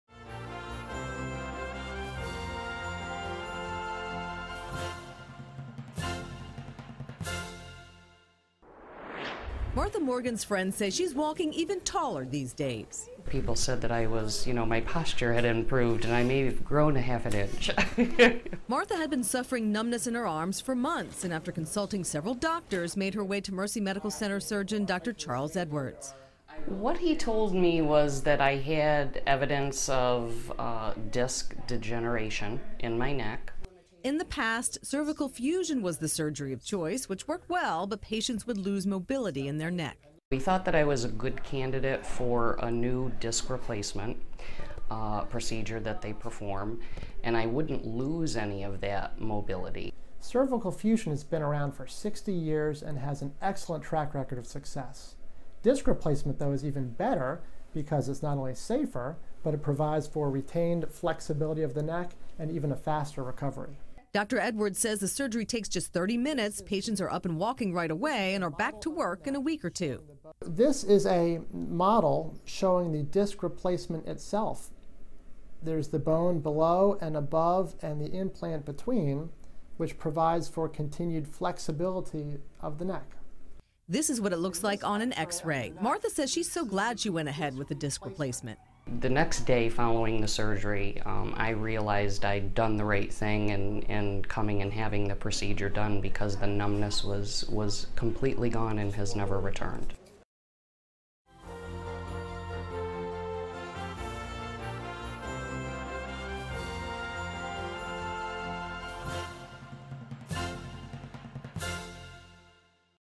Cervical Disc Replacement - Dr. Charles Edwards, II - Mercy Medical Center
Dr. Charles Edwards, II, Medical Director of The Spine Clinic at The Maryland Spine Center at Mercy Medical Center, discusses the advantages of cervical disc replacement for degenerative disc disease.